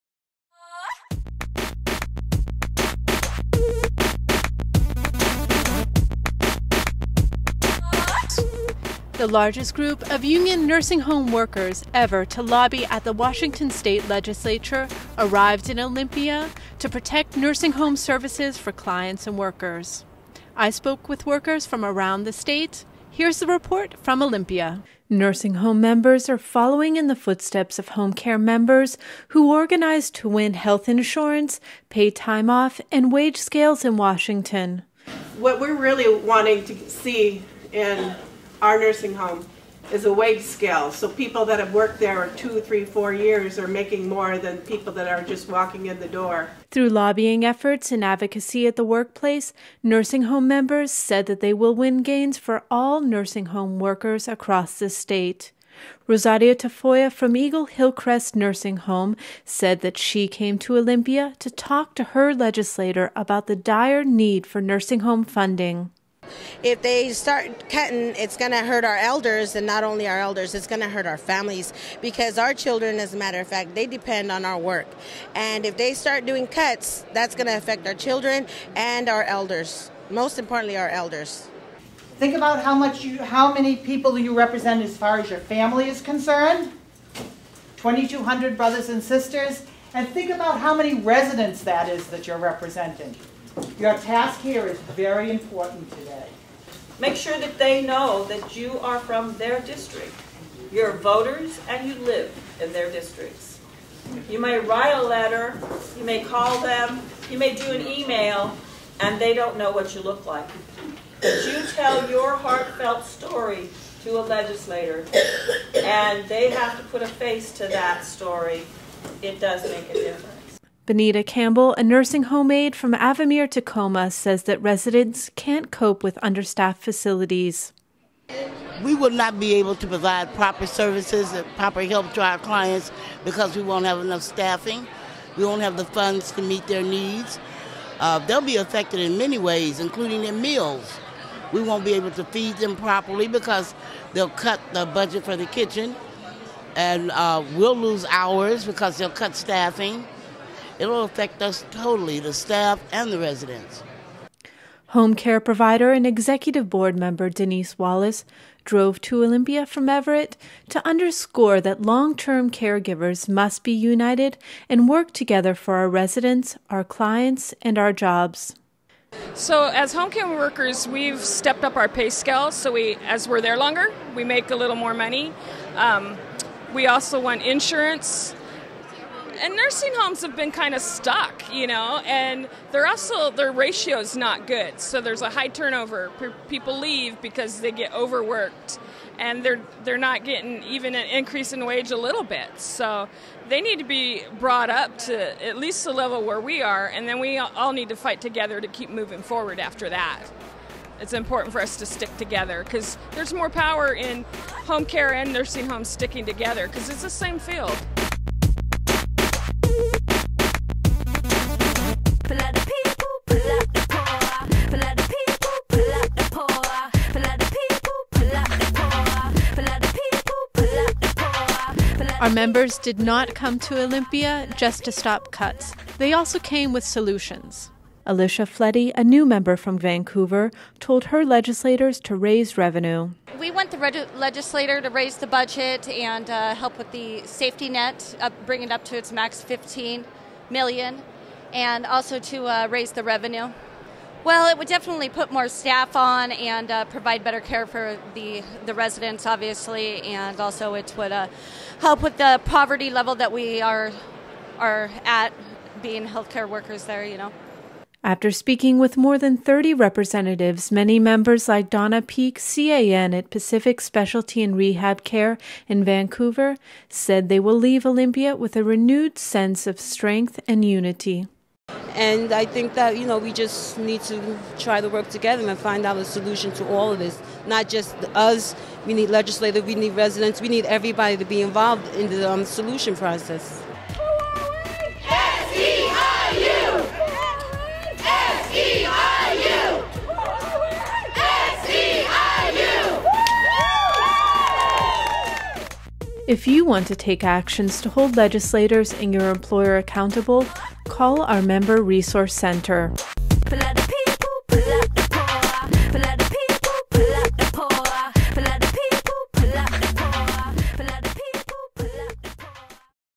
Nursing Home Lobby Day 2012
The largest groups of unionized nursing home workers ever to lobby the Washington legislature descended on Olympia Feb. 9, demanding an end to long-term care cuts and increased funding for nursing homes.